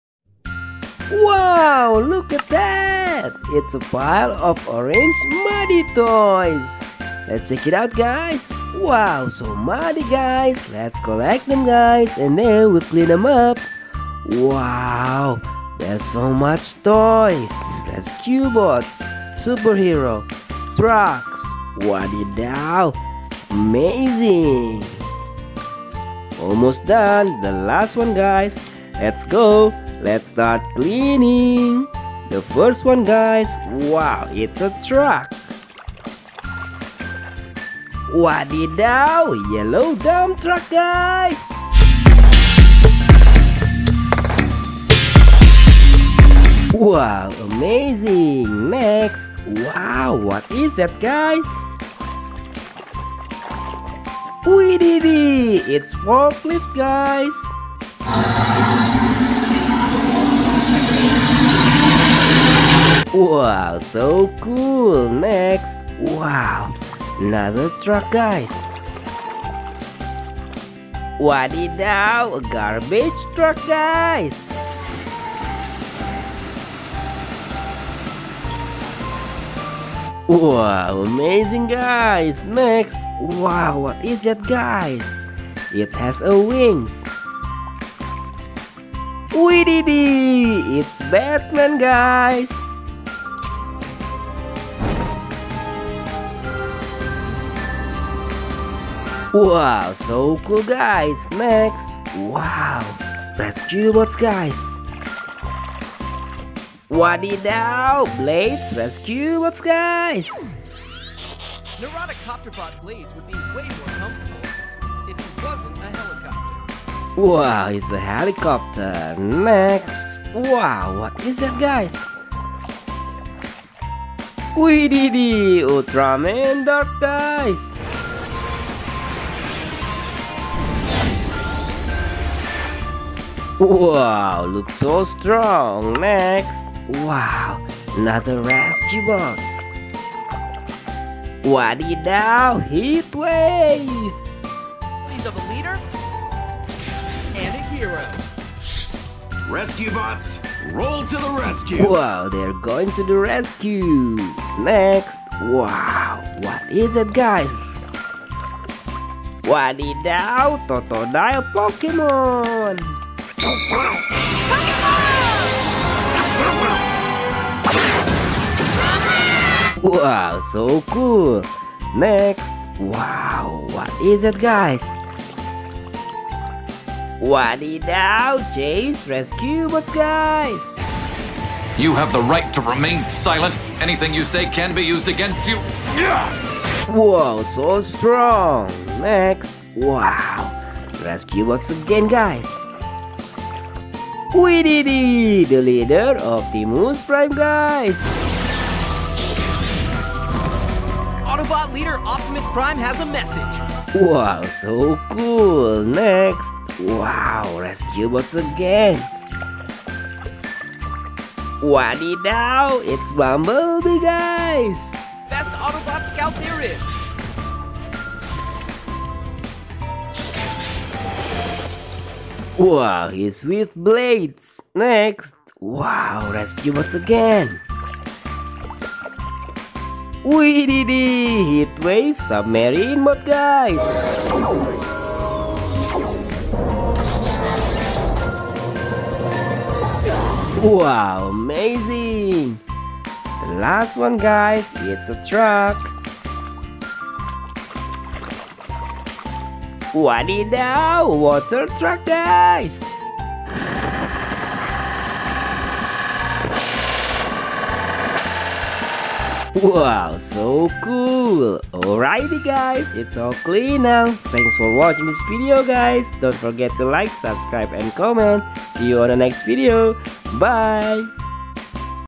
Cleaning Muddy Car Toys Batman, Transformers, Rescue Bots, Dump Truck, Forklift, Water Truck
Cleaning muddy car toys.
I hope you guys enjoy watching my videos.

Thankyou.